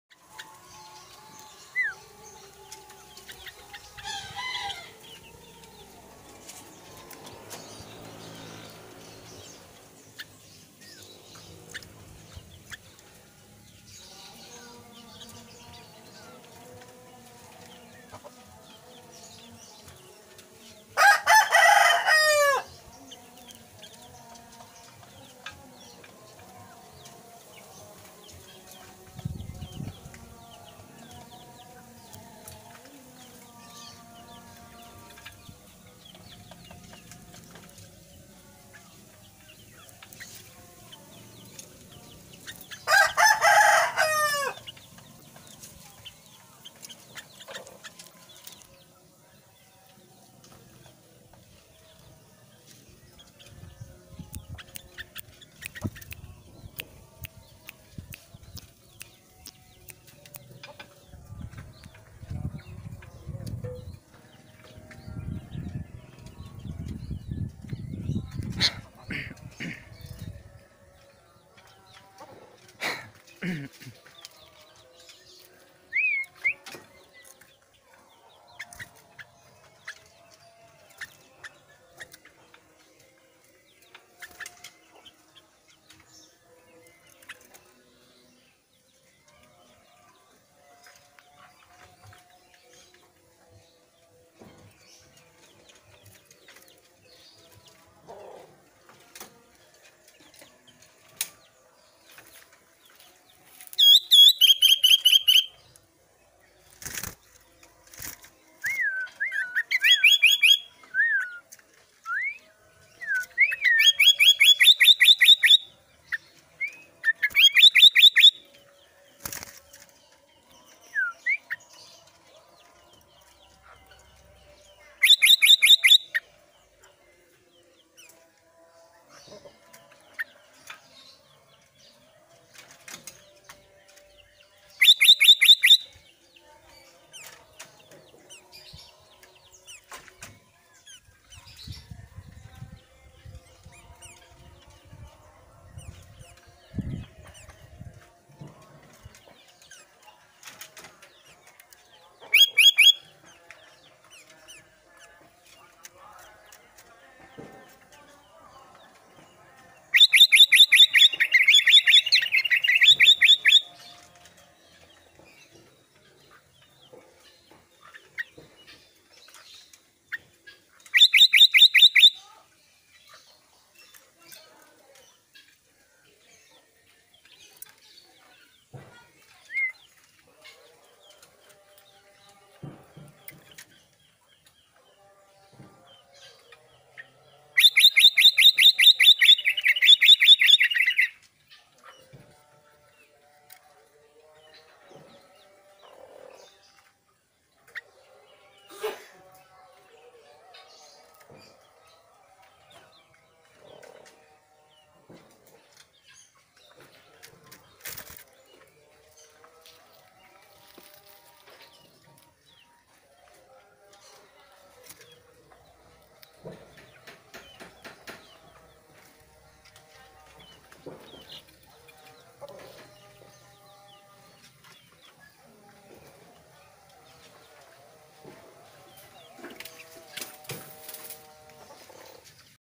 Kicauan si putih kutilang albino gacor
burung kutilang albino ini sekarang mau ber bunyi dan beberapa hari dan mau berbunyi lagi maka dari itu saya aploud ke youtube lagi